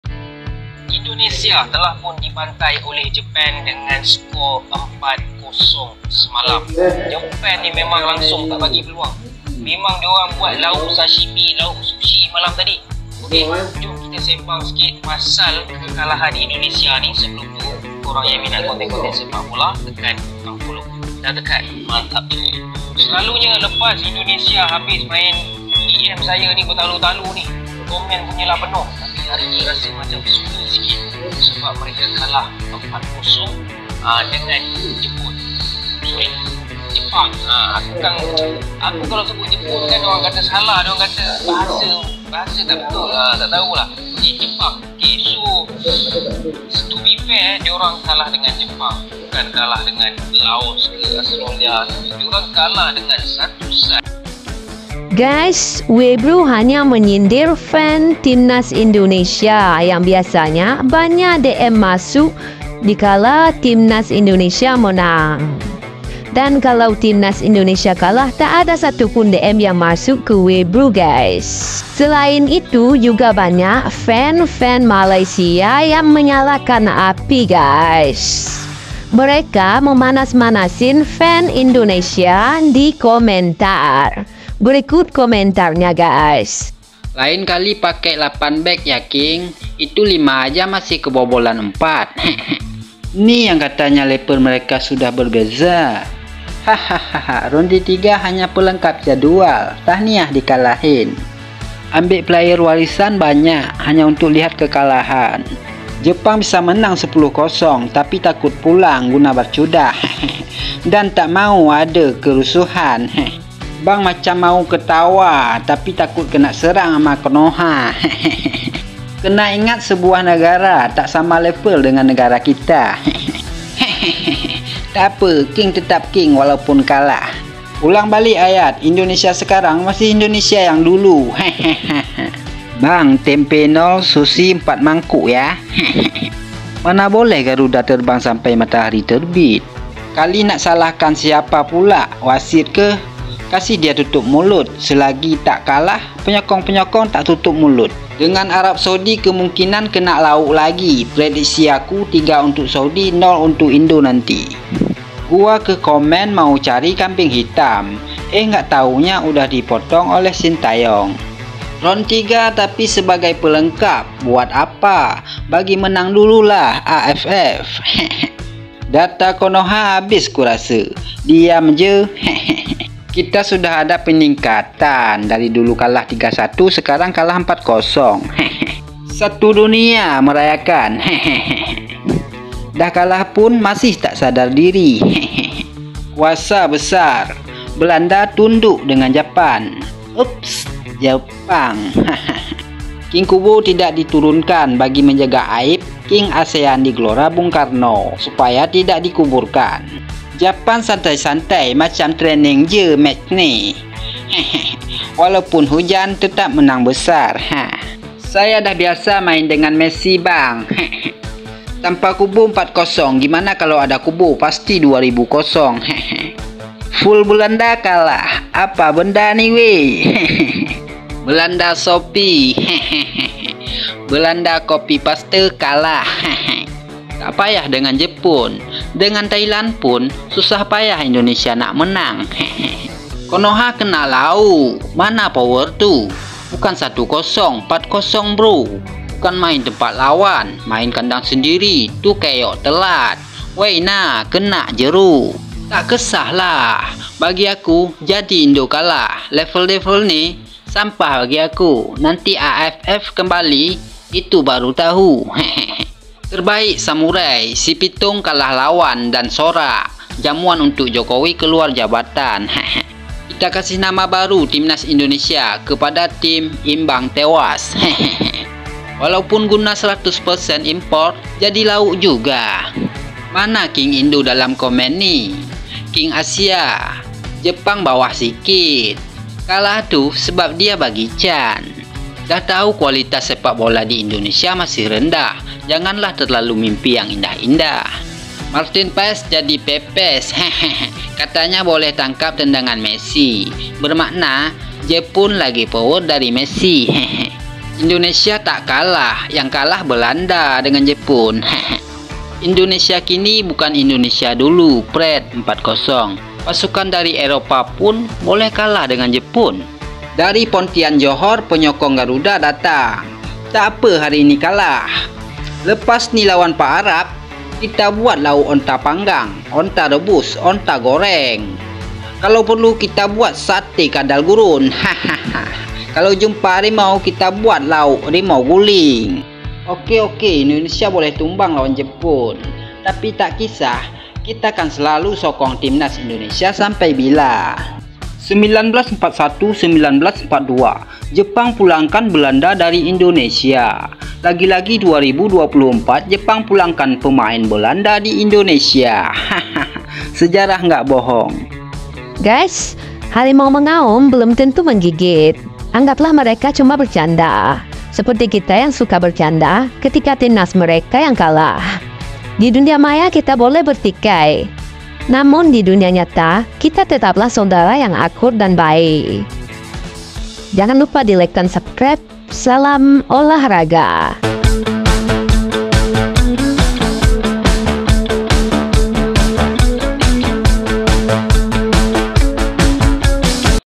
weebro sindir fans Indonesia: Netizen Malaysia kompak komporin fans timnas Indonesia!
Weebro sindir fans Indonesia: kata weebro kalau timnas Indonesia kalah tak ada DM yg masuk.
Ya, weebro biasa menerima DM tatkala timnas Indonesia menang, fans timnas menyuruh weebro untuk mereview laga timnas Indonesia, ketika kalah dengan jepang tak ada yang DM weebro, makanya dia menyindir para fans timnas Indonesia.